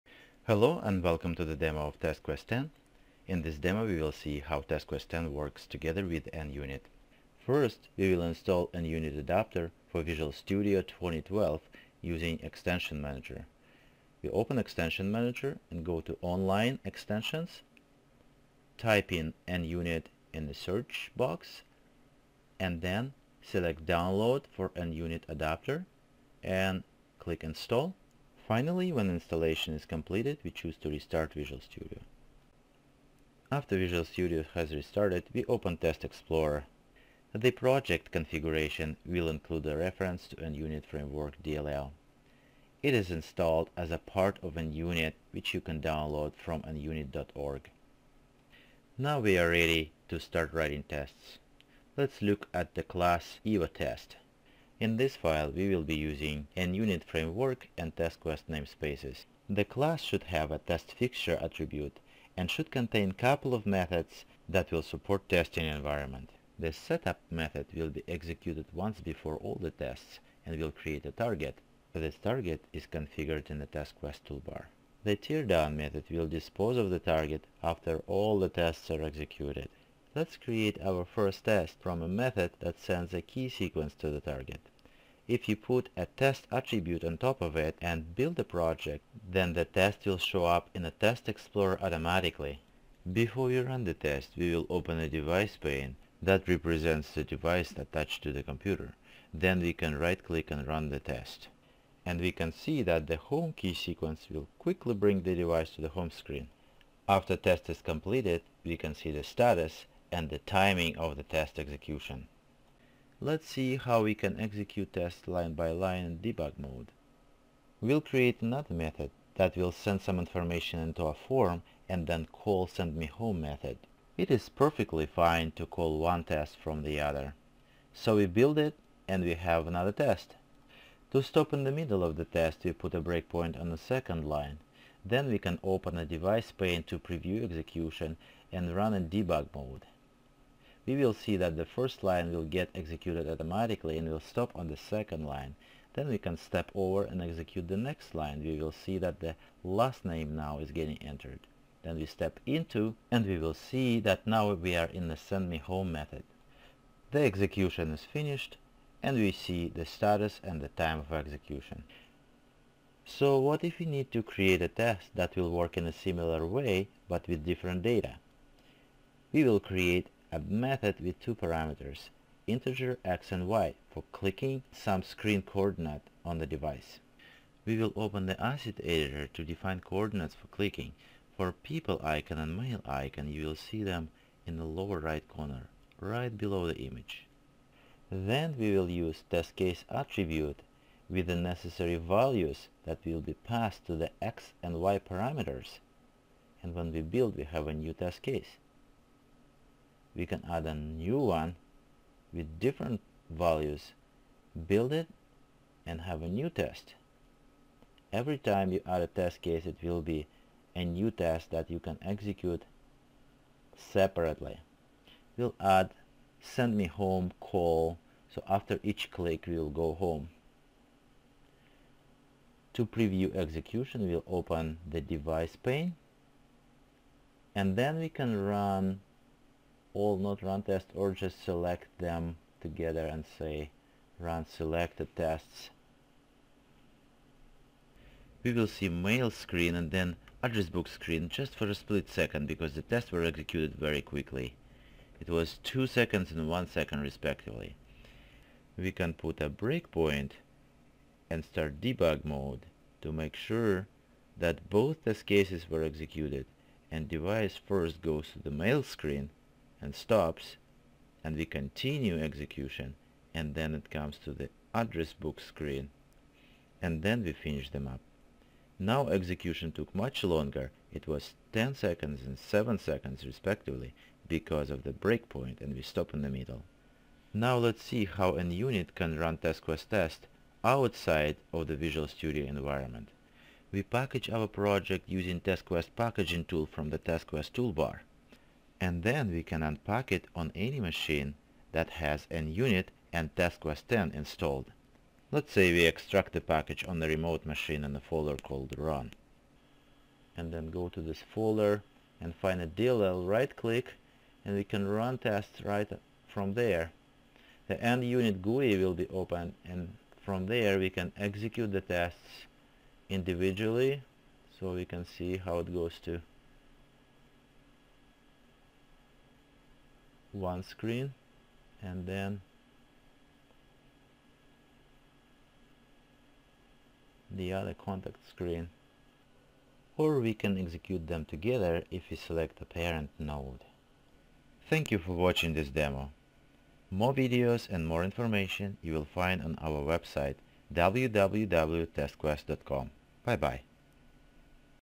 TestQuest 10 Demo - Integration with NUnit
TestQuest 10 is the premier test automation solution designed for ISVs and OEMs who want to bring applications and smart, connected devices to market quickly, at low cost and with increased quality.

This demo highlightsTestQuest 10's ability simplify development and test efforts by integrating with NUnit.